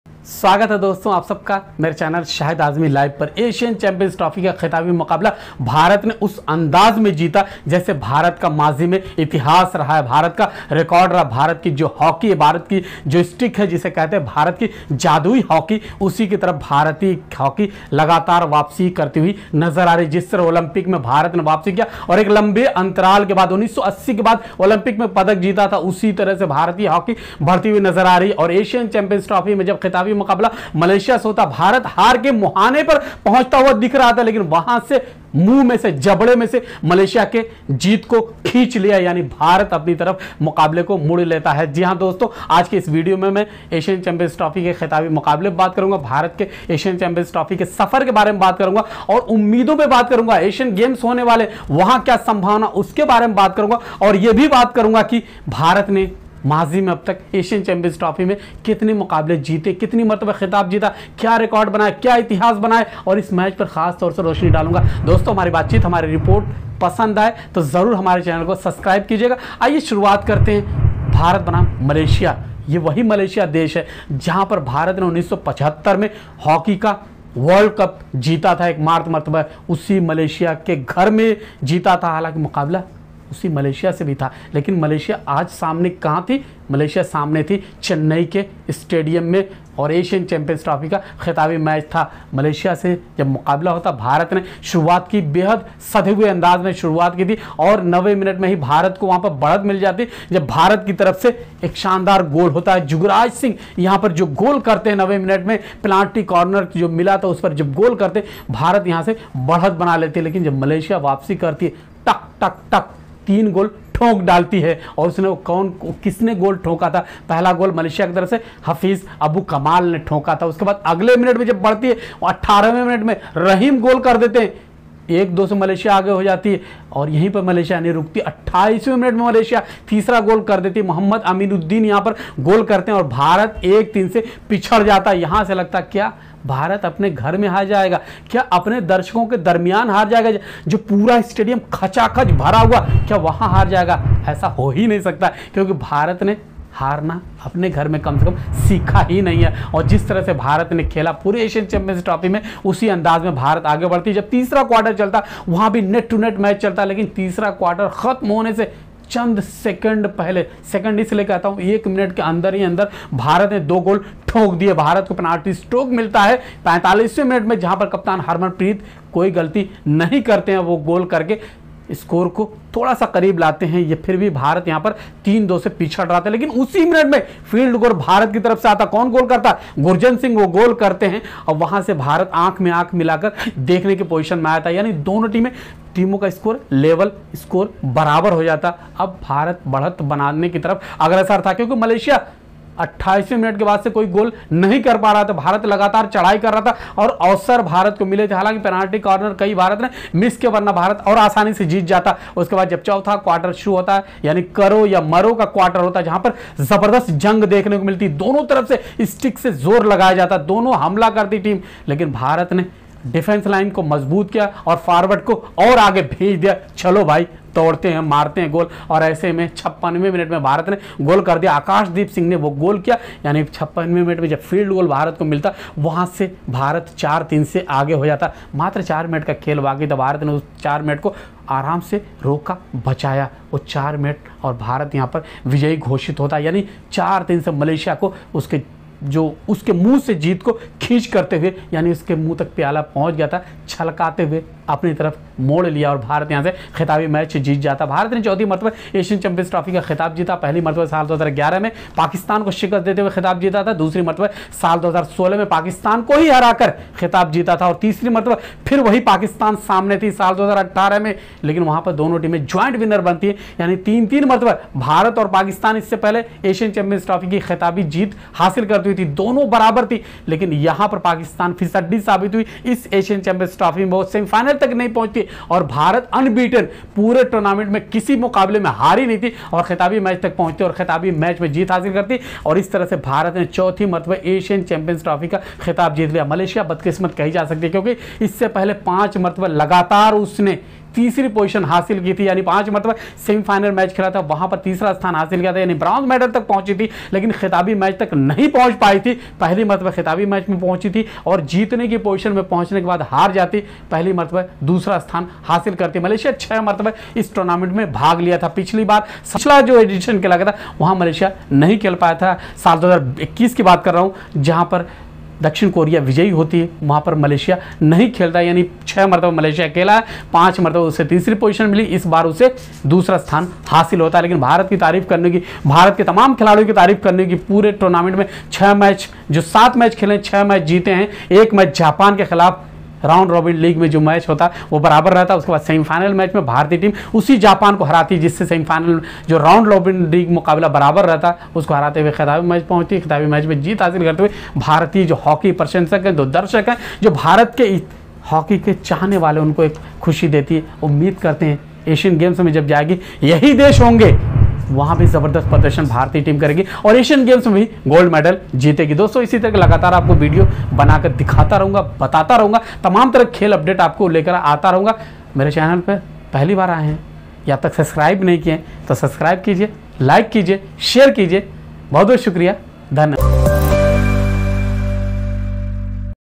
Highlights India vs Malaysia Final | Asian Champions Trophy | Hockey
Highlights India vs Malaysia Final | Asian Champions Trophy |hockey | Hockey |

India is back at the top of the hockey, returning to glorious hockey days

India - Road to Final
asian Champions Trophy

india beat China 7-2

india drew Japan 1-1

india beat Malaysia 5-0

india beat Korea 3-2

india beat Pakistan 4-0

india beat Japan 5-0